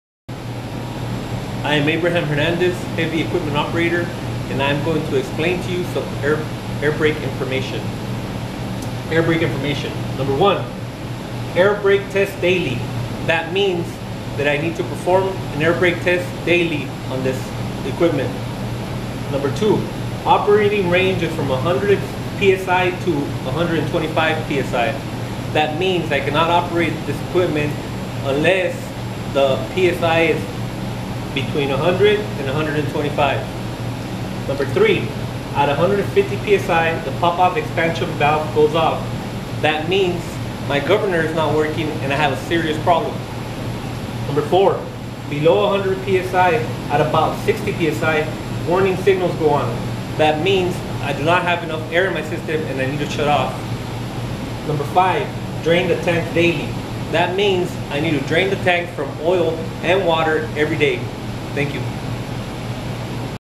Air Brake Information with HEO Abraham Hernandez
Here i am explaining some important air brake information.